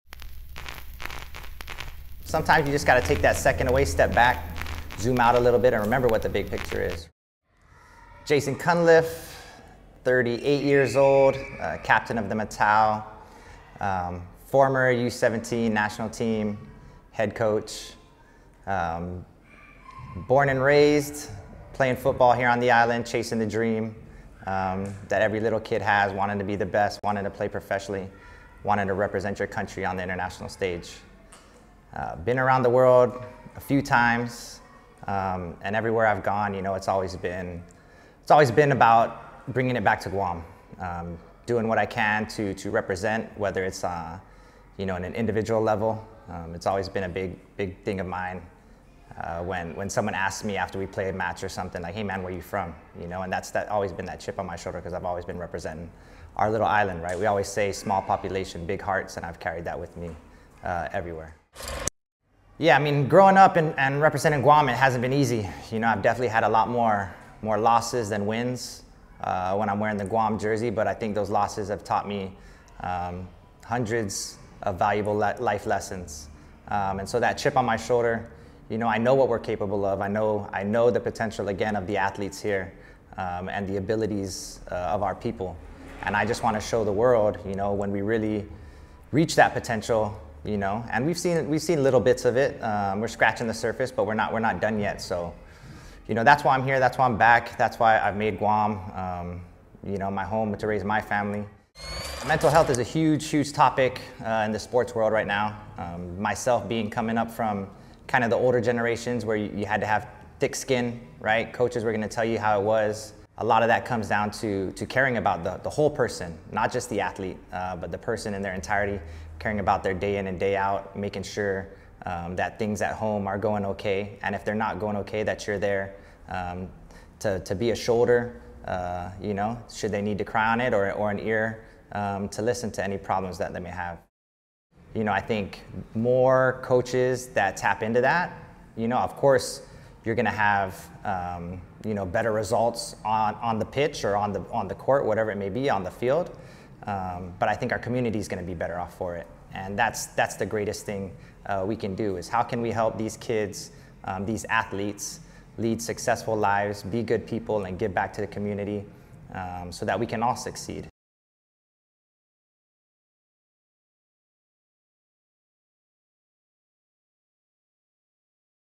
More than Sports | Jason Cunliffe
Clutch Guam presents "More than Sports." An 8 part series featuring 8 local athletes sharing their stories and perspective on mental health. This episode features the captain of the Matao (Guam National Soccer Team), Jason Cunliffe.

If you or someone you know is struggling with their mental health, please contact Guam's Crisis Hotline at (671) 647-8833/ 647-8834